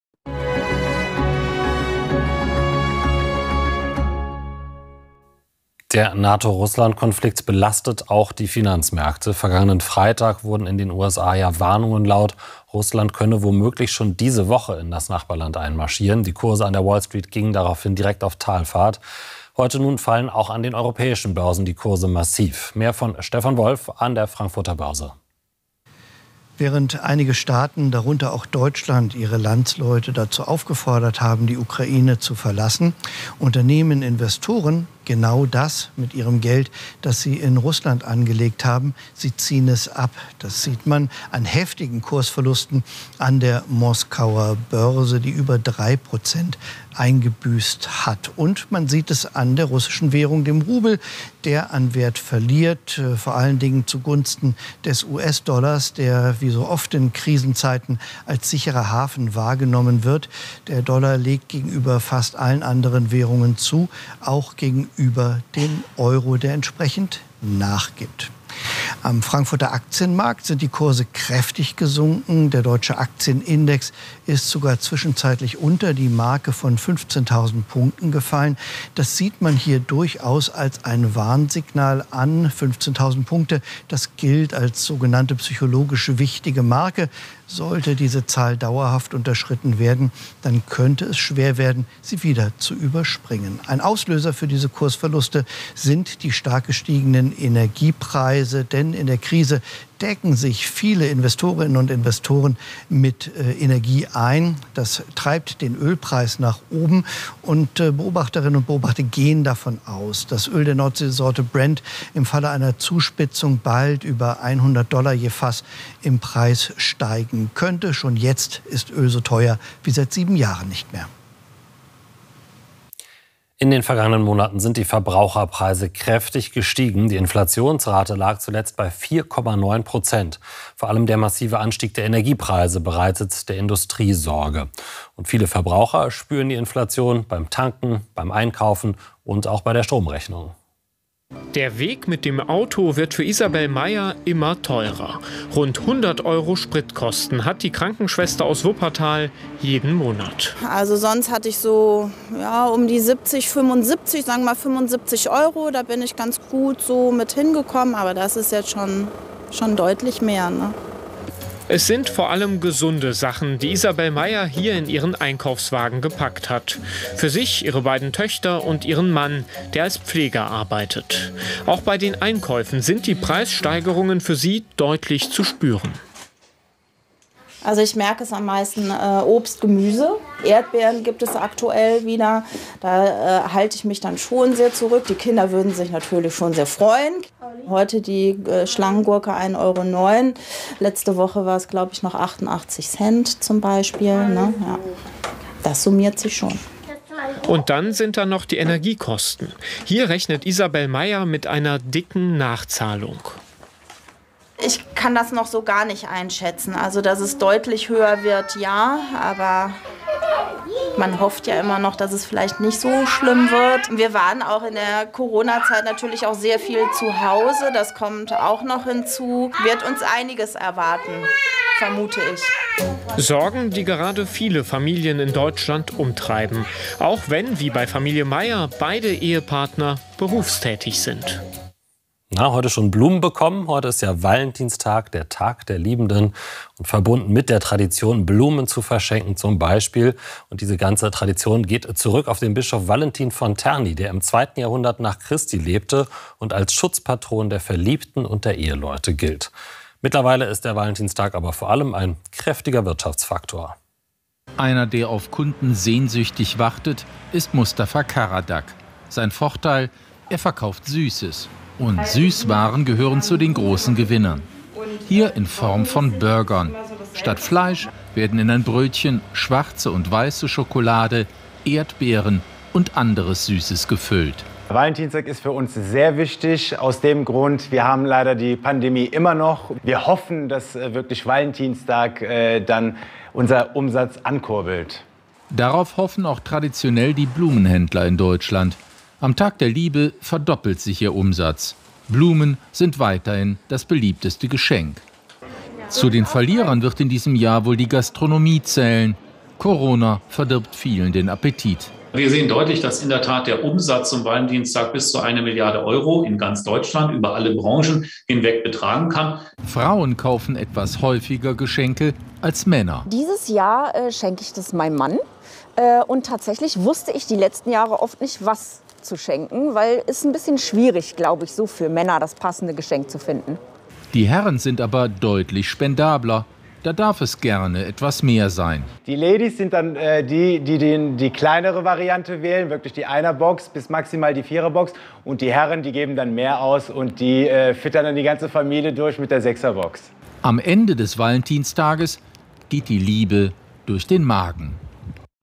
Die Börse, Hohe Inflation belastet Verbraucher, Börse in Moskau eingebrochen
WIRTSCHAFT: Die Börse, Hohe Inflation belastet Verbraucher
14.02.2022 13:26